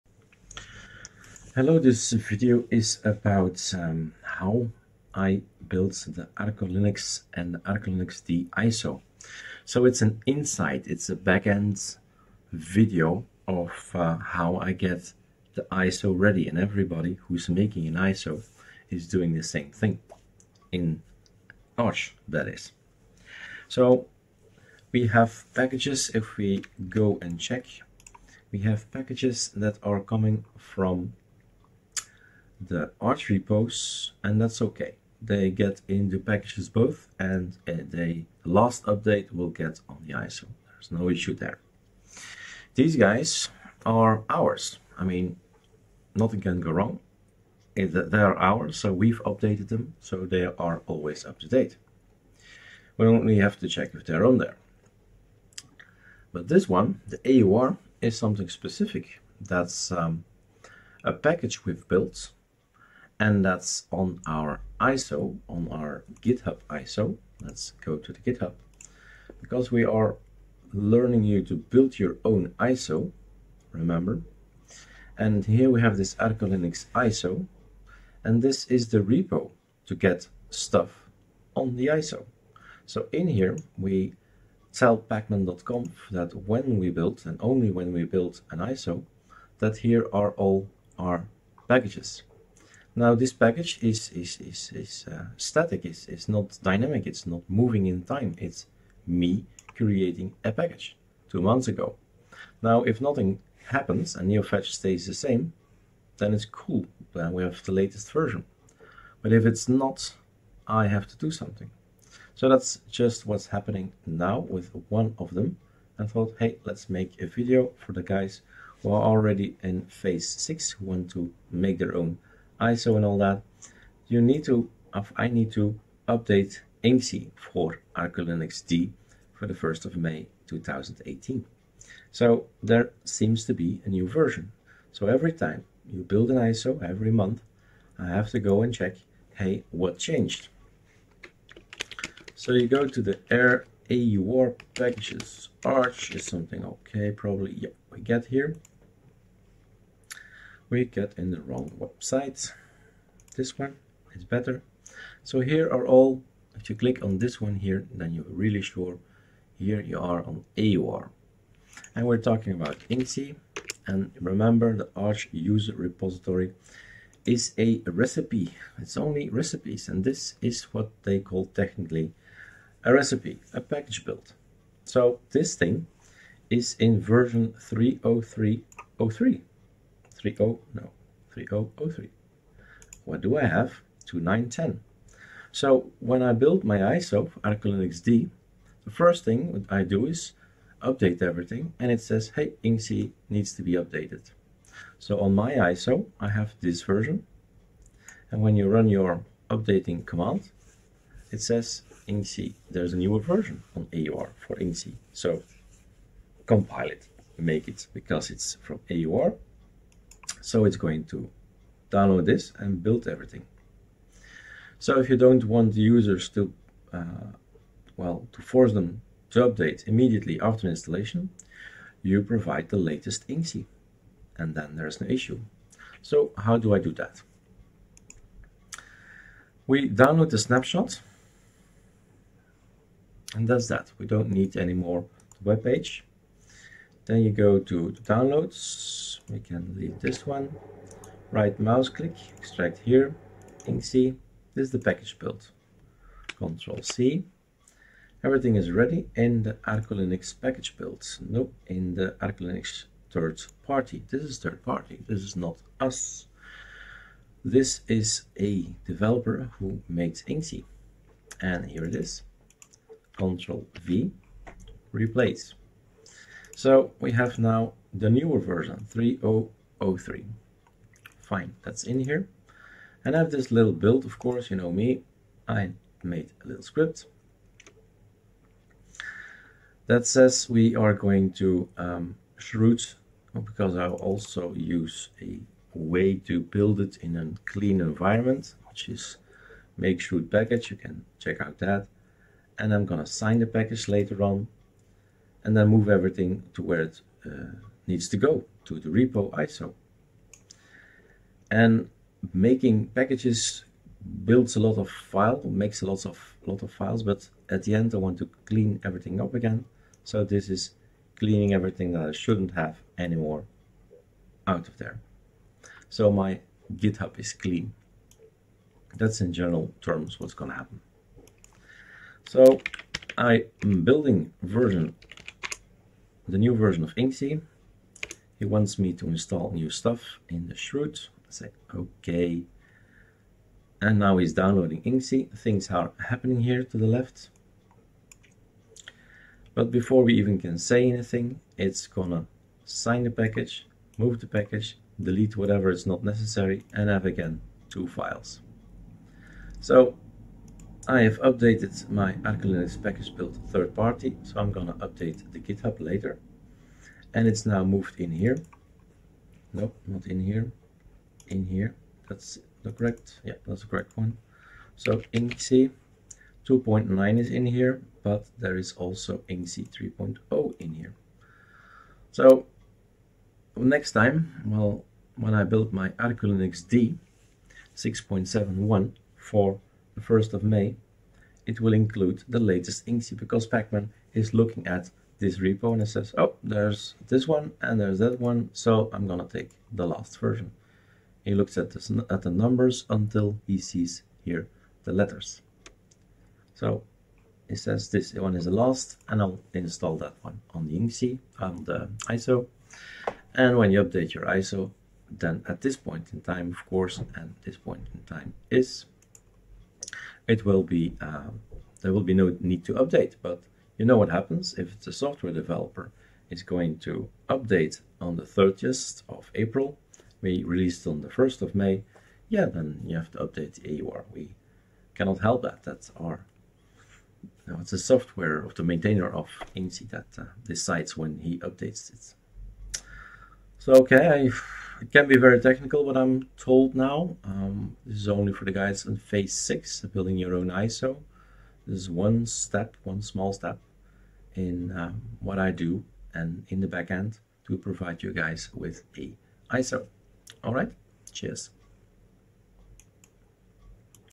ArcoLinux - How to build a package to get ready to release an iso
Insight information in the back-end how to build a pkg for the iso
Try makepkg in the folder with the PKGBUILD and you will be able to build a package.